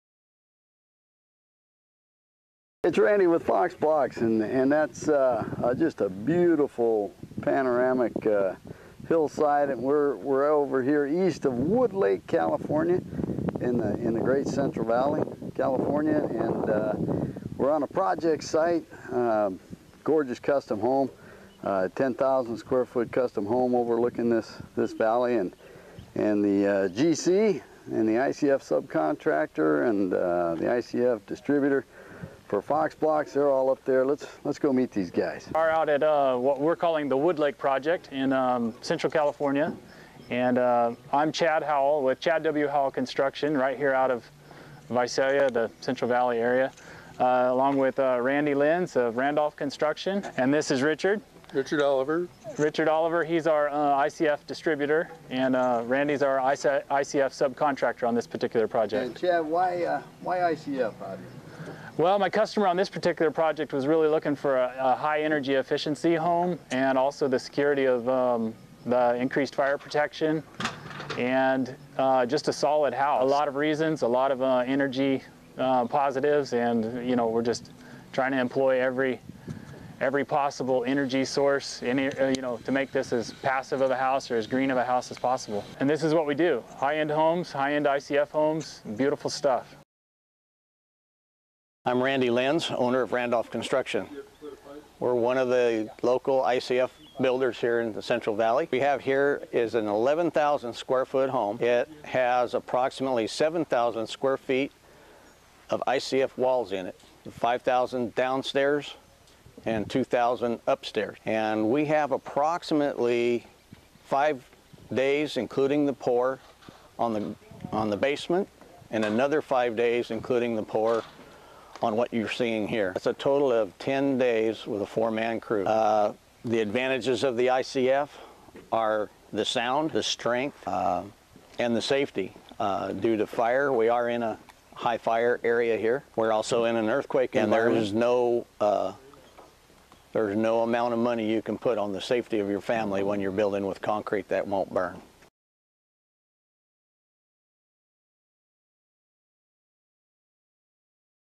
Fox Blocks Residential GC and Sub comments speed strength and safety of ICF homes
General Contractor discusses why client chose ICF. ICF Contractor discusses facts and figures on man hour rates with Fox Blocks.